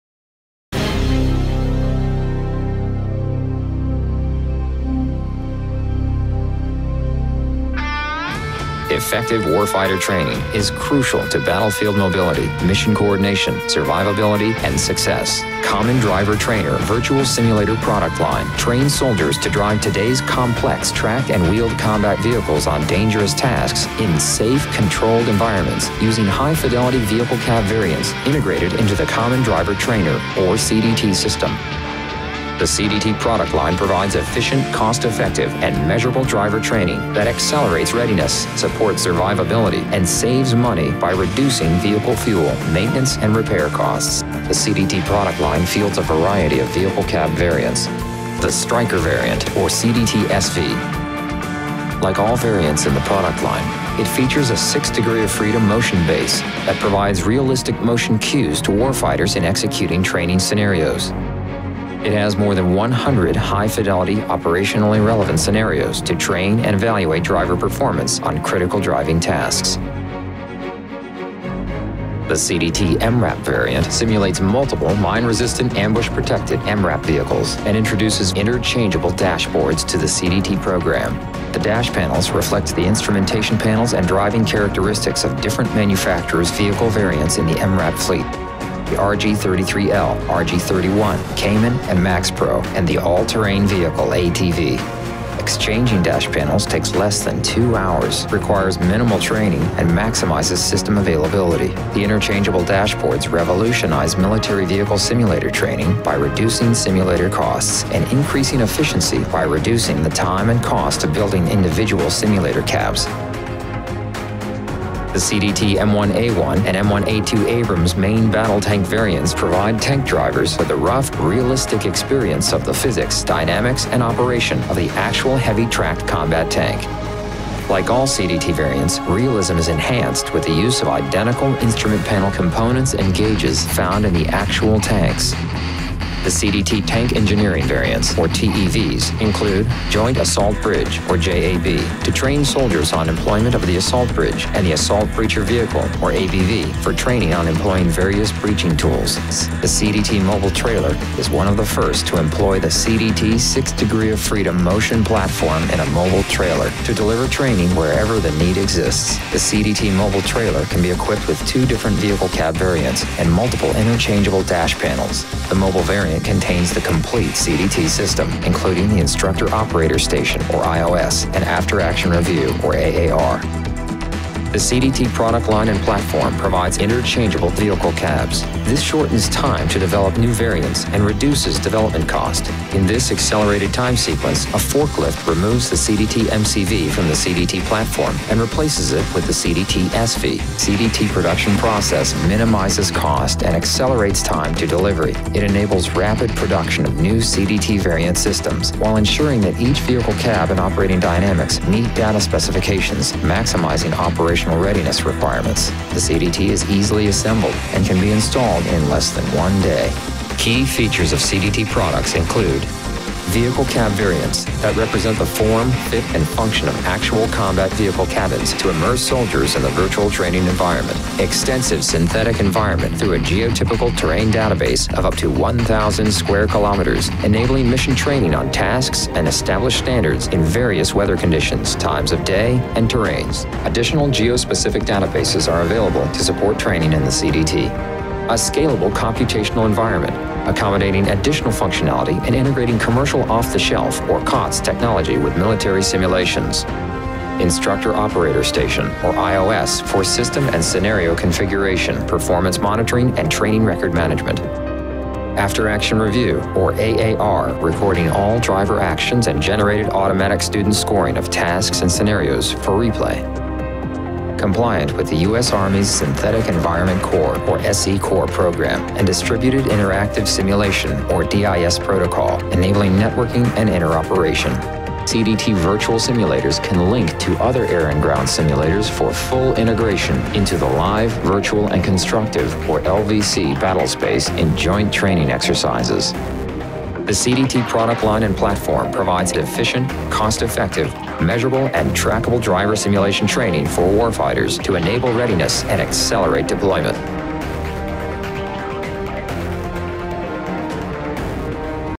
Common Driver Trainer
Understand our cost-effective product line of realistic simulators to train military vehicles crews to ensure mission readiness, survivability, and success.